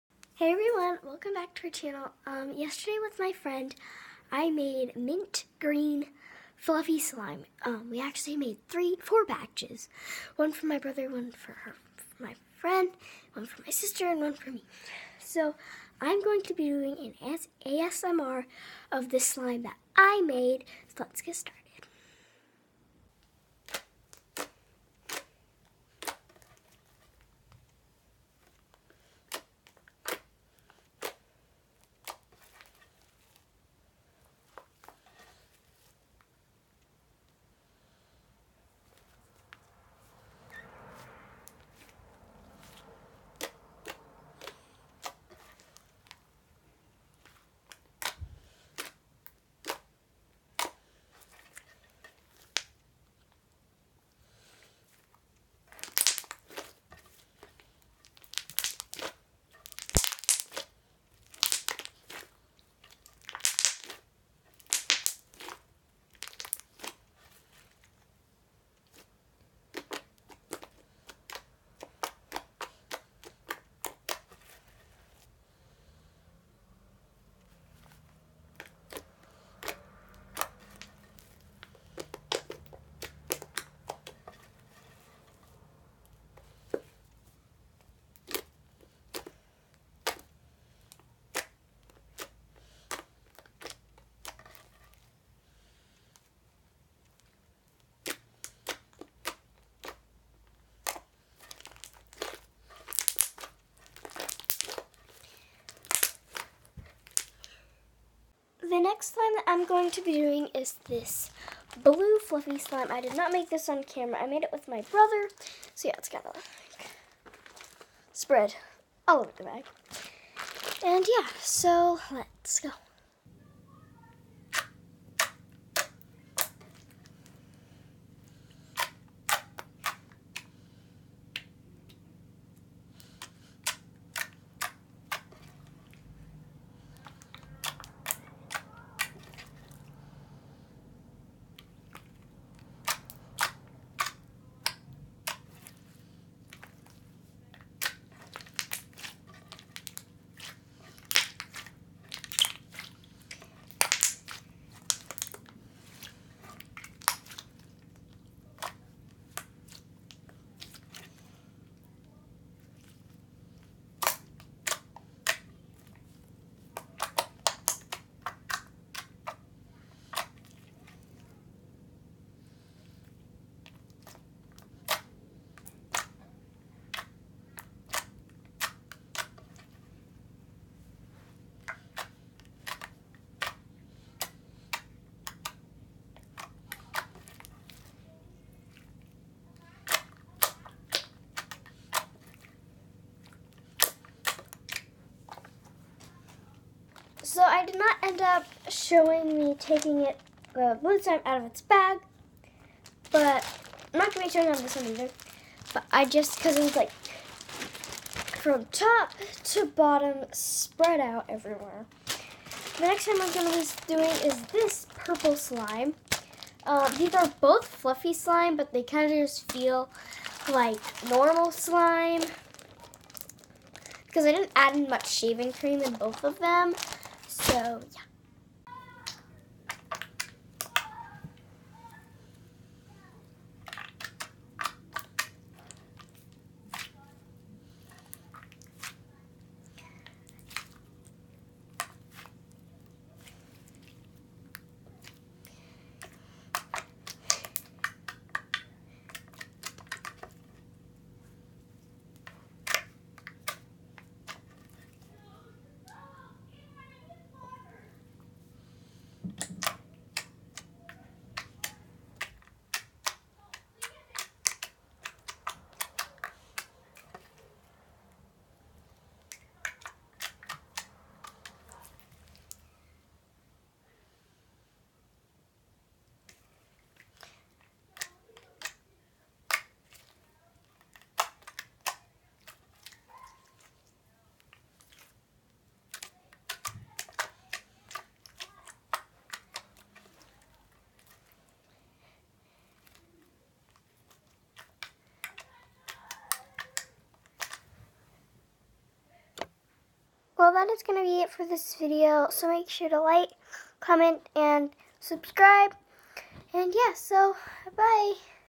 Slime ASMR!
Today I poked, squished, and played with 3 different slimes!